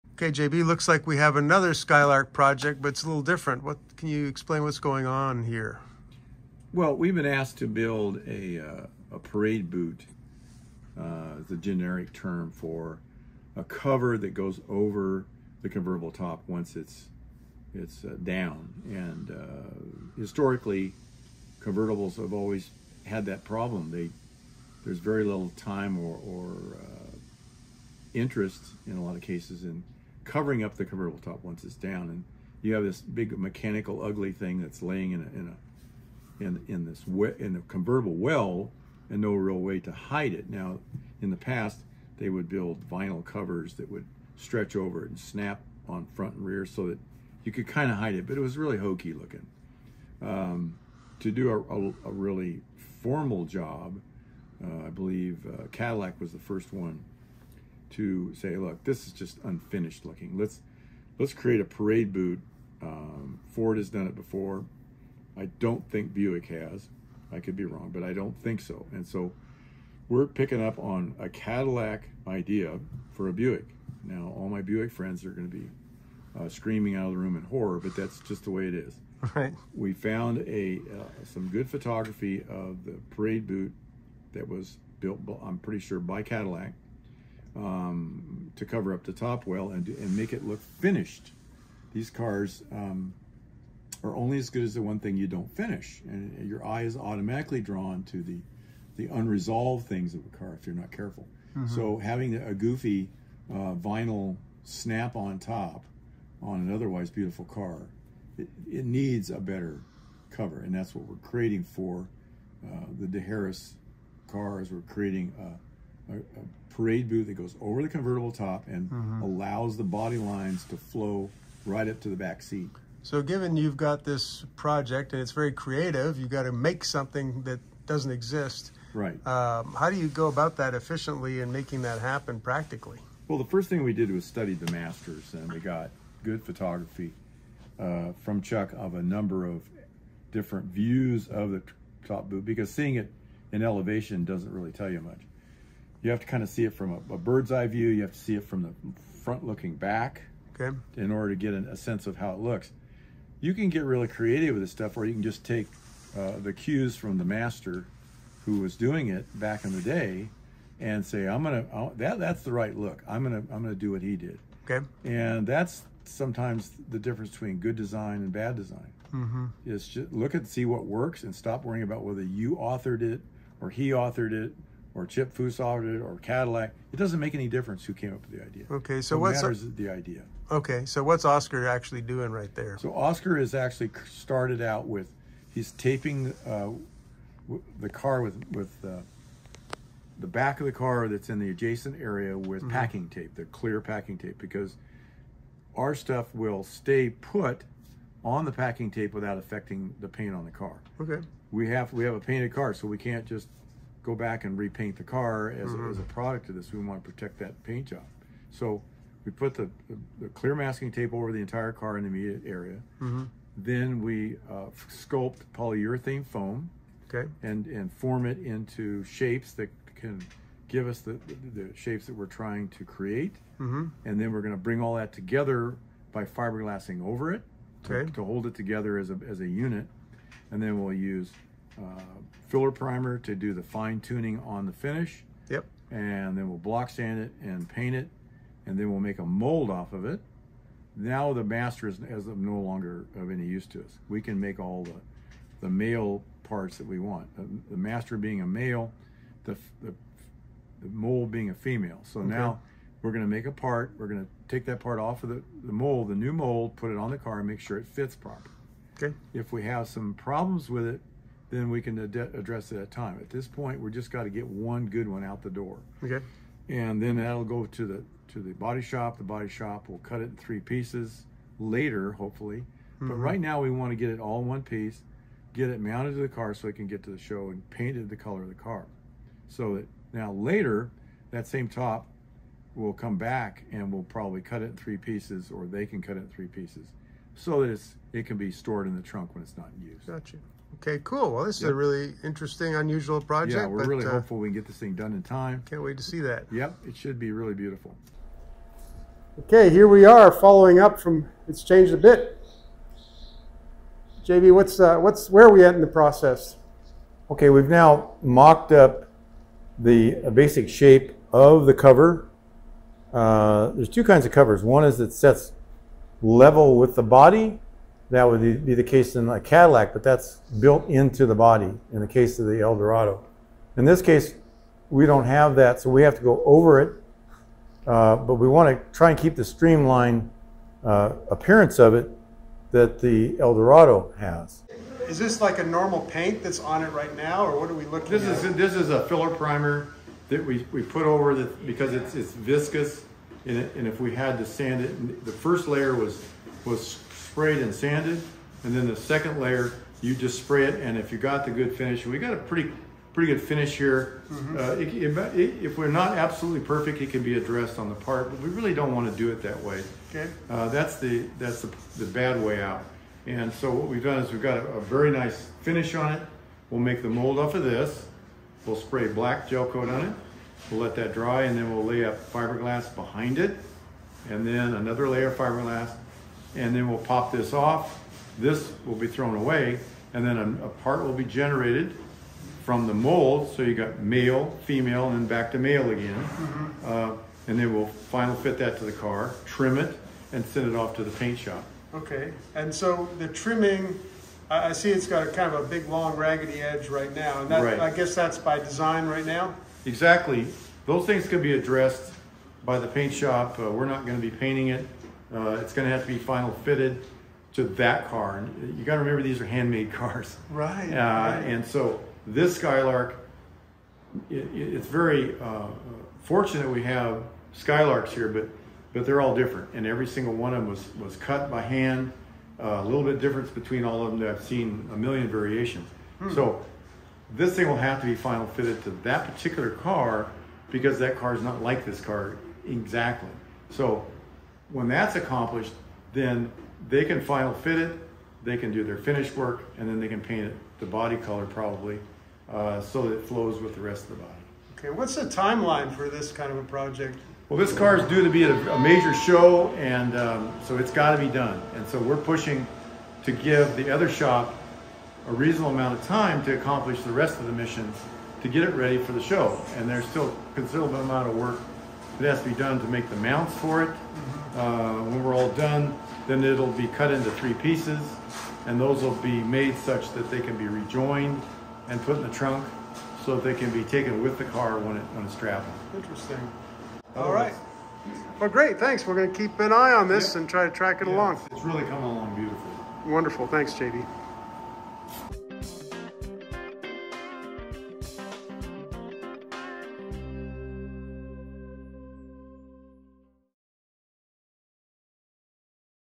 1953 Buick Skylark Convertible by Chuck DeHeras. JB's team creates a custom parade boot.
JB Donaldson now creating a parade boot for the convertible top for the Chuck DeHeras Buick project underway.

project or parts inquiries contact American Fiberglass in Phoenix AZ